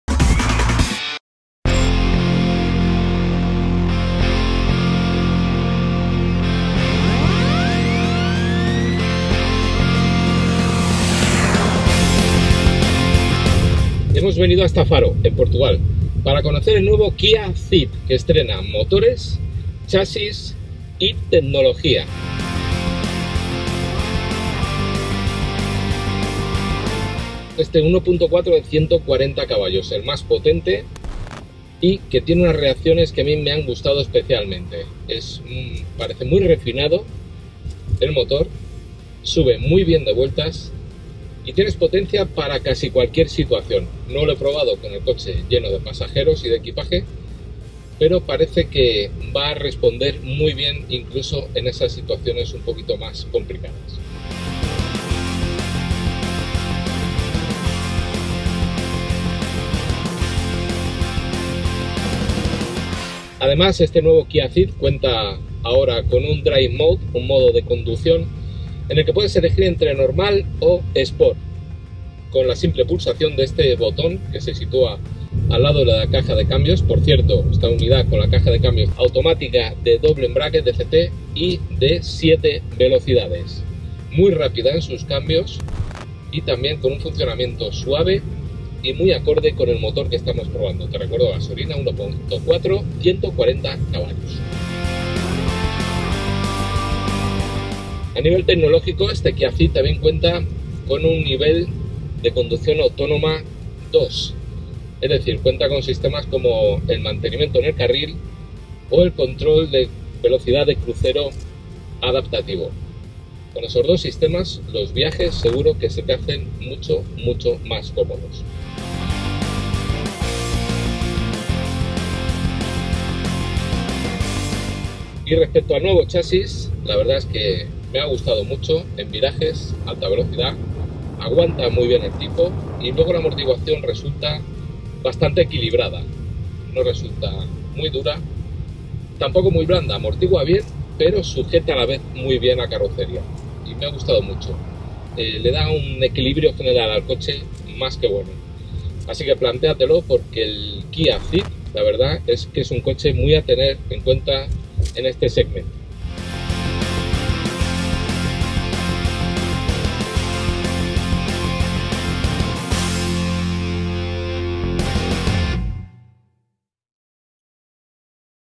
Kia Ceed 2019, primera Prueba / Review / Test
A modo de resumen te diré que el nuevo Kia Ceed llega con un exterior de líneas más rectas, más calidad de materiales a bordo, una nueva plataforma denominada K2 (que comparte con su primo-hermano el Hyundai i30) y algunas mecánicas nuevas. Entre estas últimas hay dos nuevas diésel de 1.6 litros y 115 o 136 CV, así como una de gasolina 1.4 de 140 CV, que es la que puedes ver en este vídeo.

Además, su nivel de conducción autónoma alcanza el nivel 2, gracias a su control de crucero adaptativo y a su asistente de seguimiento de carril. Pero aquí no acaba la tecnología que incorpora el Kia Ceed 2019: cargador inalámbrico para tu Smartphone, conectividad total con él por supuesto, equipo de sonido JBL, asistencia para el aparcamiento, alerta de tráfico cruzado trasero, etc…